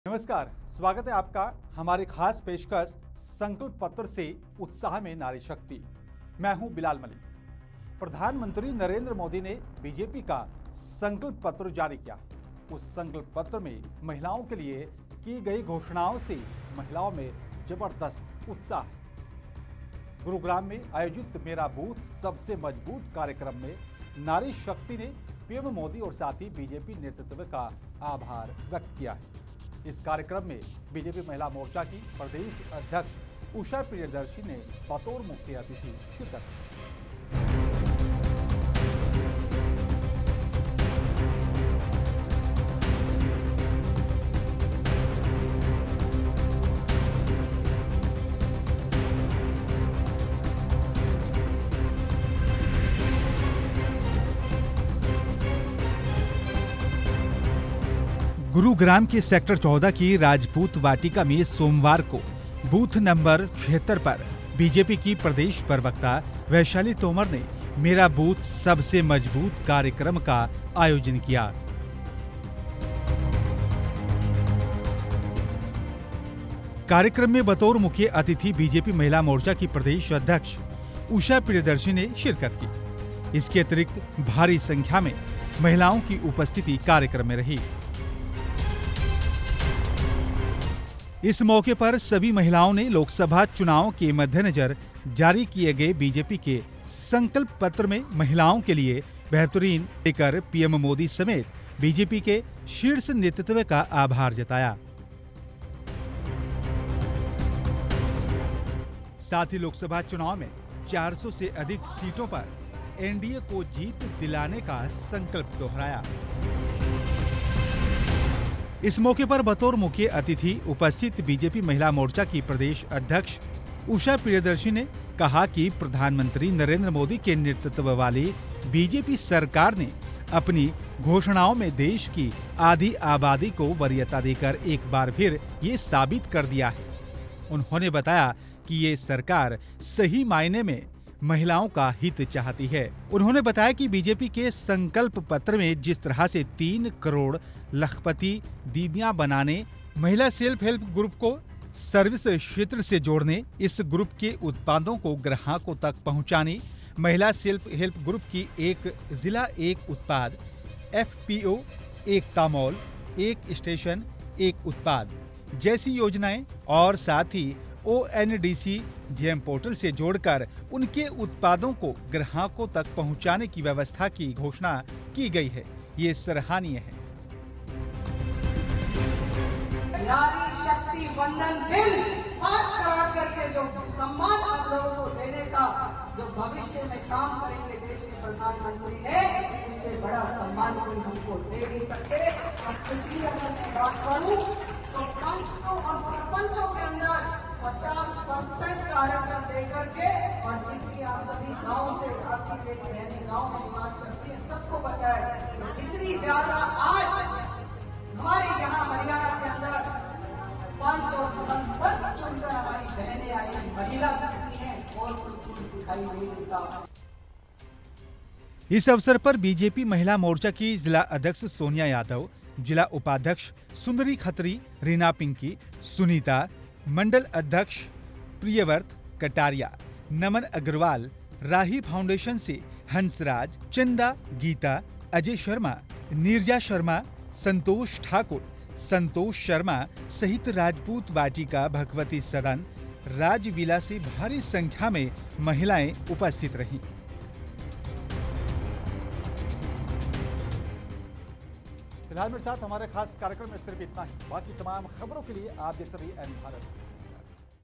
संकल्प पत्र से नारी शक्ति का बढ़ा उत्साह, BJP महिला मोर्चा प्रदेशाध्यक्ष उषा प्रियदर्शी का बड़ा बयान
'संकल्प पत्र' से नारी शक्ति का बढ़ा उत्साह...', BJP महिला मोर्चा प्रदेशाध्यक्ष उषा प्रियदर्शी का बड़ा बयान | Gurugram | N Bharat